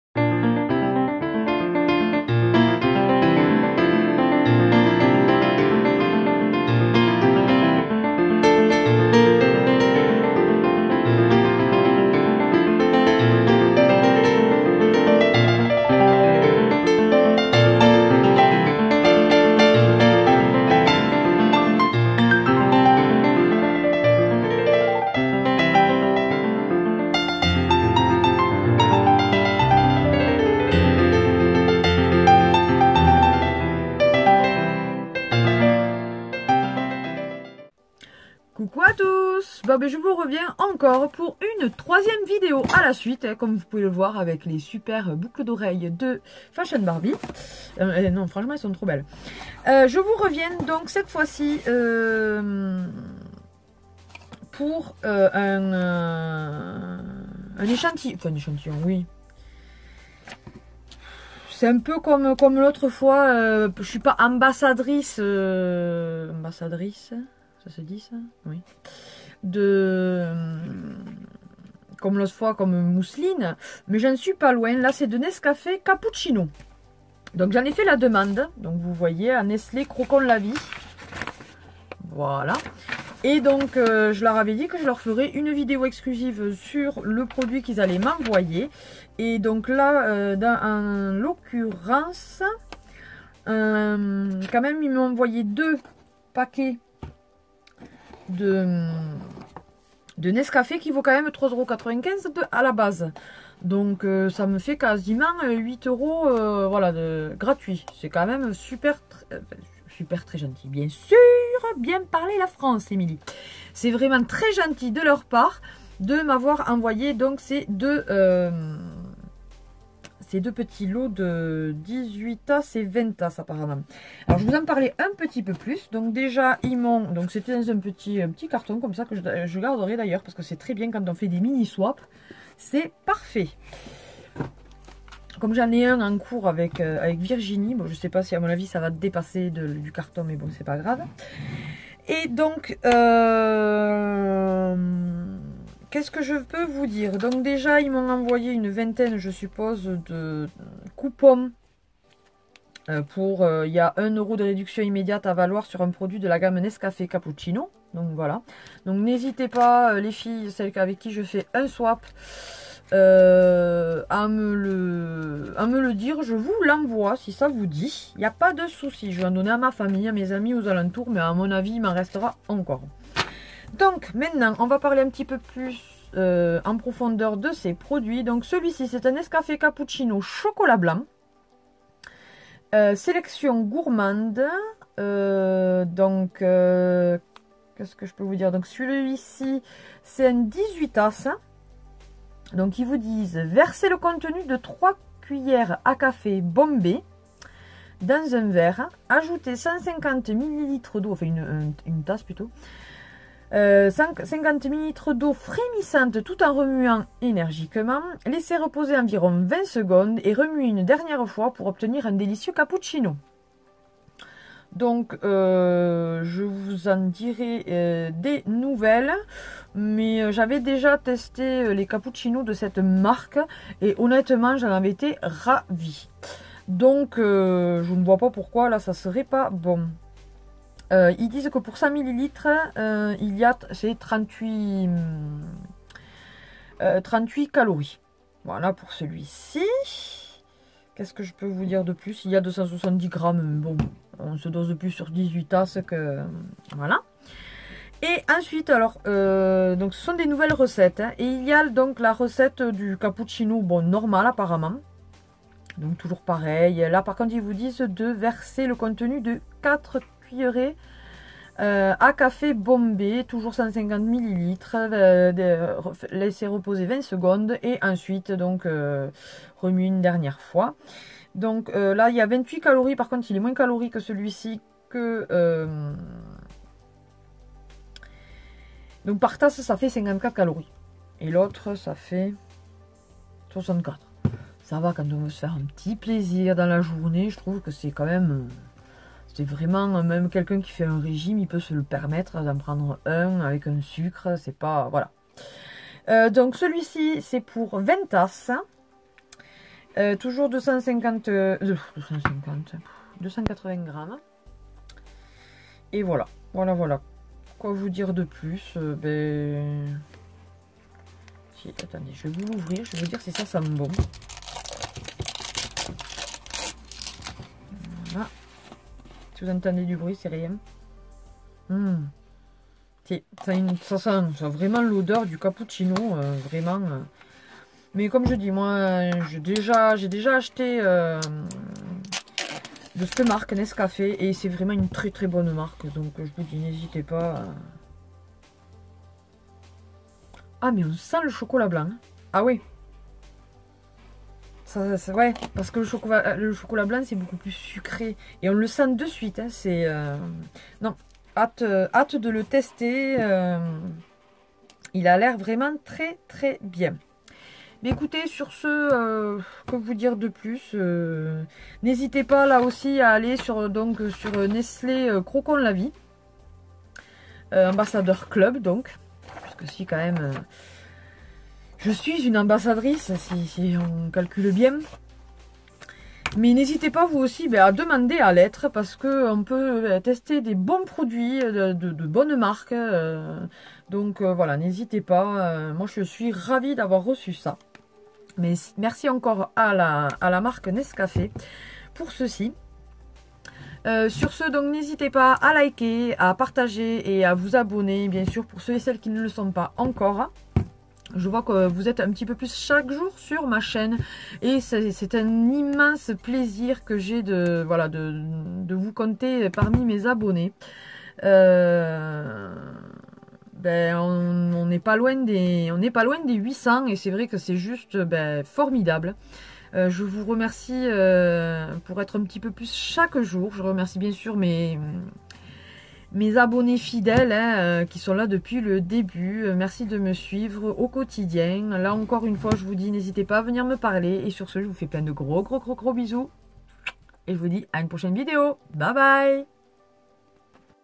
Je suis maintenant ambassadrice Nestlé lol
On m'arrête plus lol maintenant je suis ambassadrice de Nestlé à  quand pour un produit makeup lol
N'hésitez pas à partager liker et vous abonnés de gros bisous mes chéris
Mon Facebook